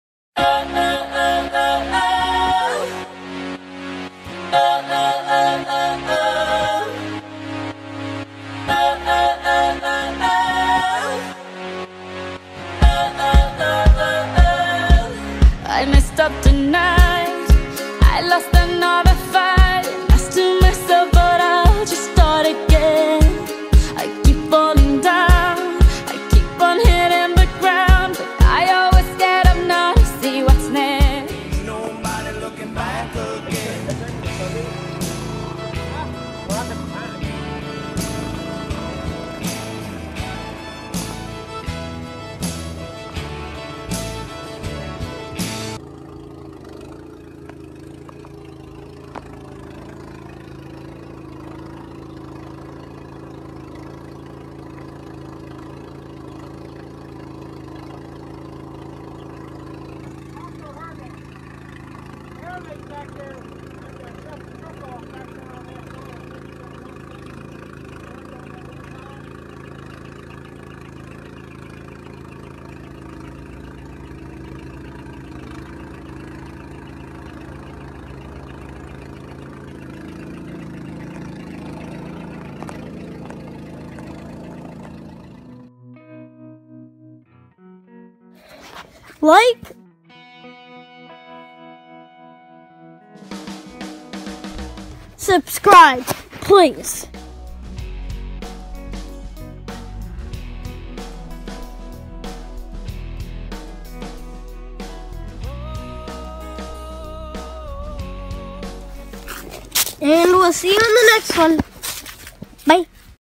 Trash guy getting air in his flat tire. “OLD VIDEO”
I make videos of garbage trucks and vacuums! i am also a team with Crazyguys 123